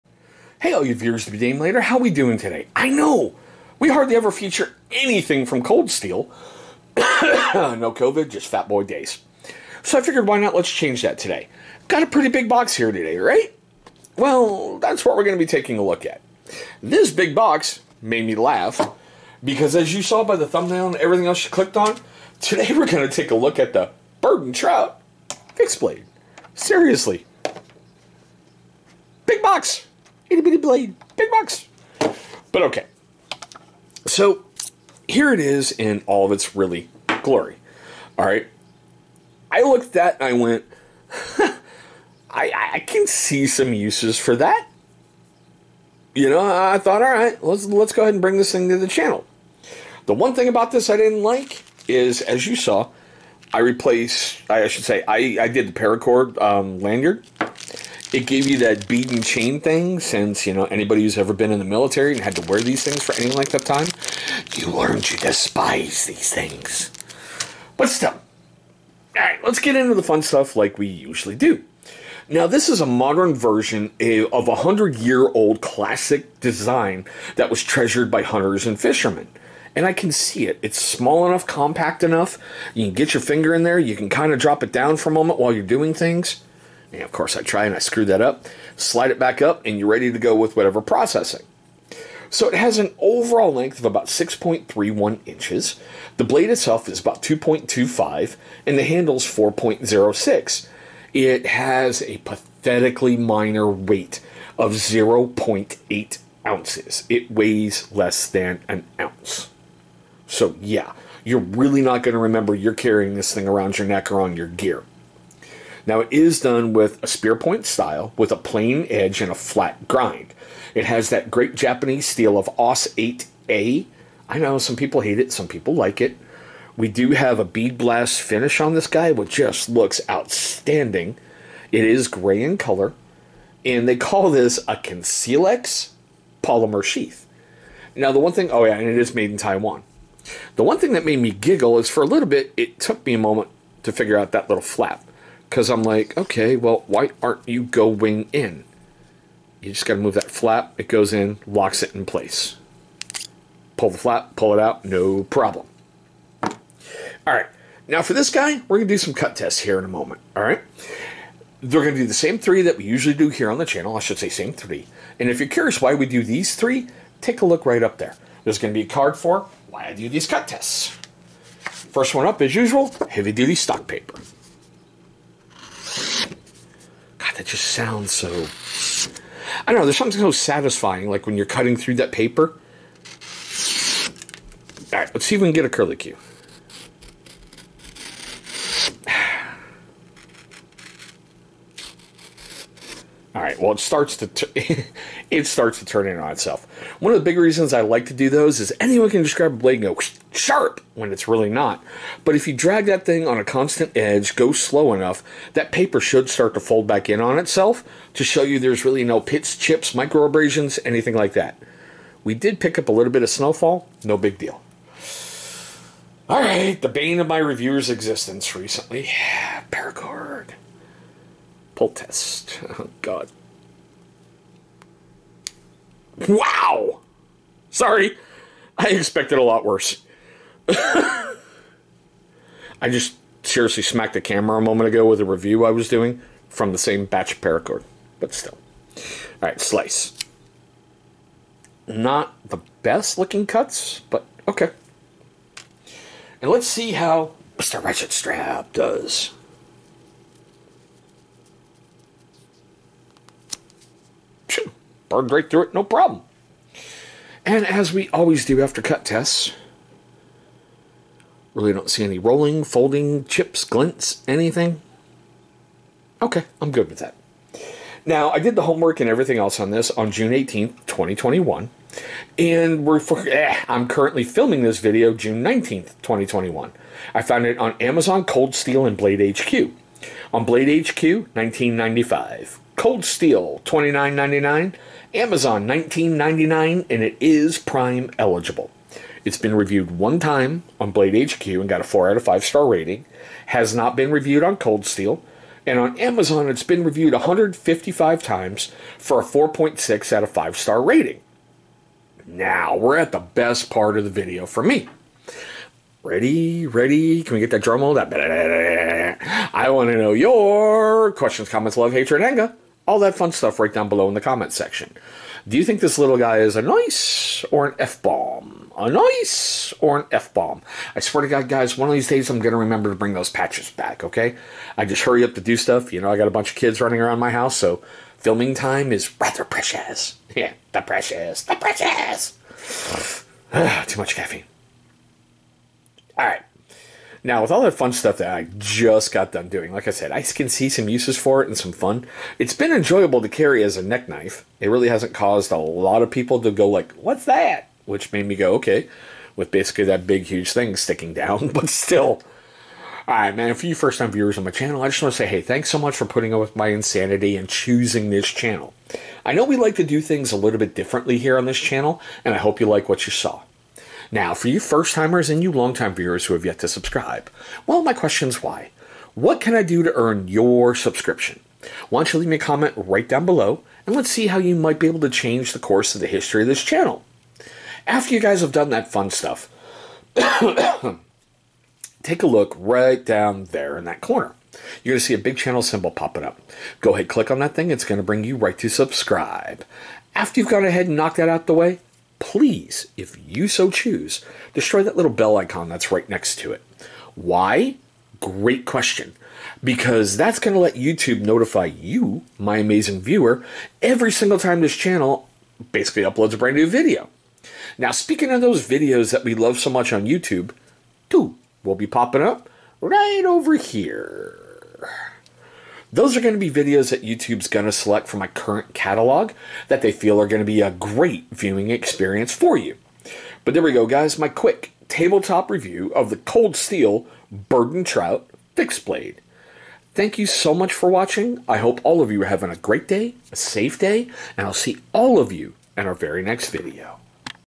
Cold Steel Bird and Trout Fixed Blade
There was something about this blade that peaked my interest. Other than the fact I haven't taken a look at a lot by Cold Steel. But today on the channel we have the Cold Steel Bird & Trout Fixed Blade. I think this might be a great addition for any hunter

If you would like to purchase this blade, I have left a link to amazon.com